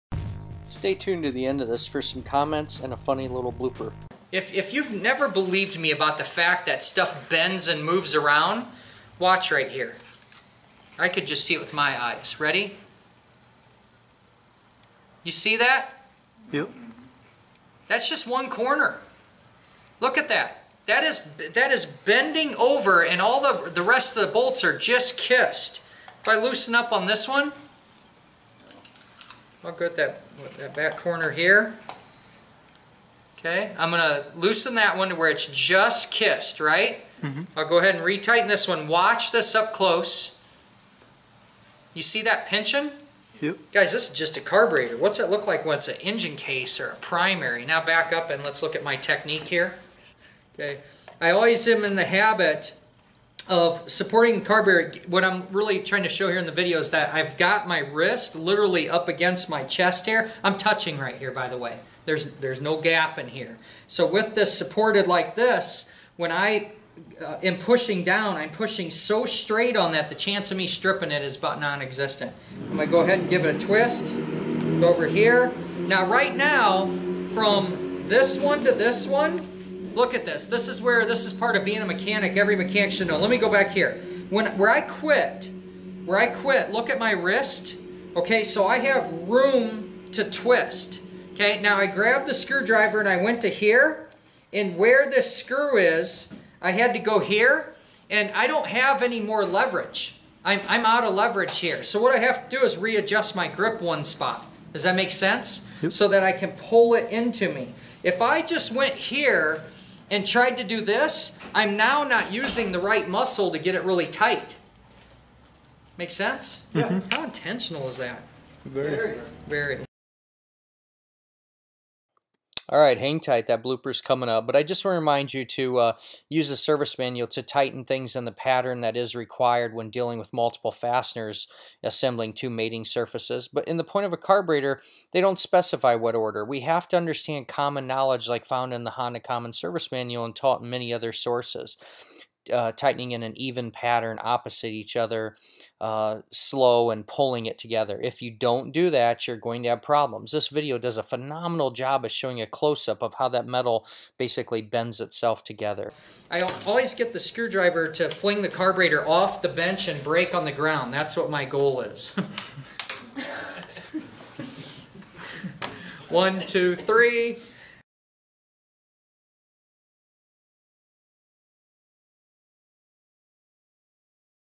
EM Series: Why you must "kiss" two mating surfaces together! Carb Bowl Tightening Example UP CLOSE!
Finally an up close shot of two mating surfaces flexing as you assemble them!